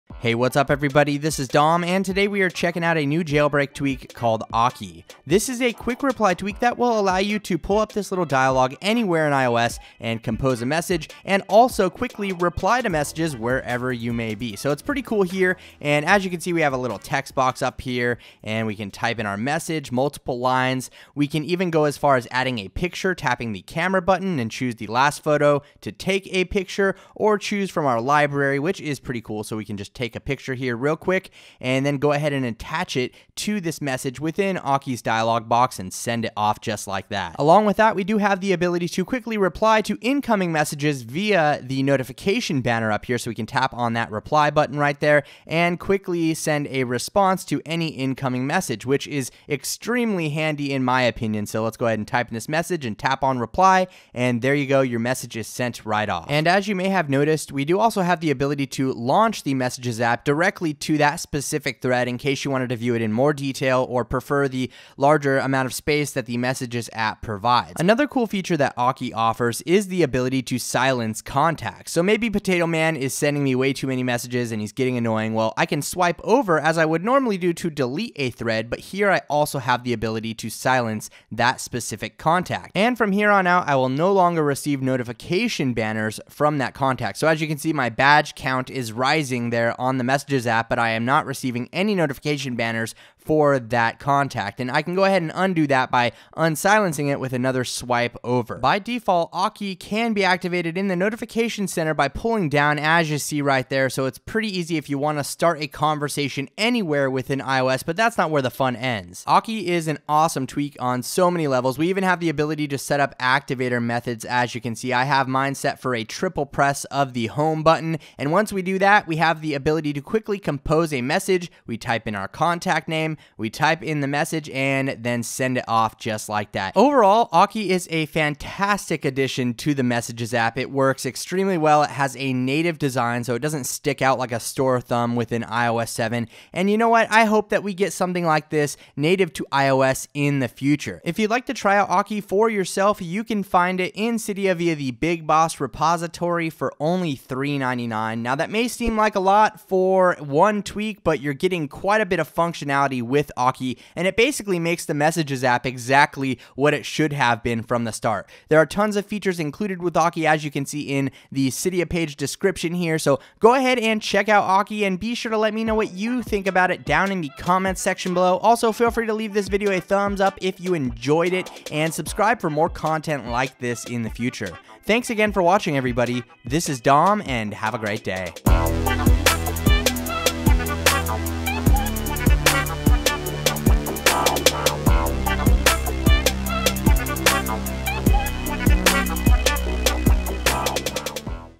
iOS 7 Jailbreak: Auki - The Best Quick Reply Tweak Ever
iOS 7 Jailbreak tweak: Auki may be the best quick reply tweak ever.
Auki - available in the BigBoss repo for $3.99 - iPhone / iPod touch.
Created by @Bensge and @Surenix (Follow them!!)
Auki is hands-down the best quick reply tweak that I've used so far. It has a native design and is a joy to use. If you'd like to check out Auki for yourself, it's available for $3.99 in the BigBoss repo.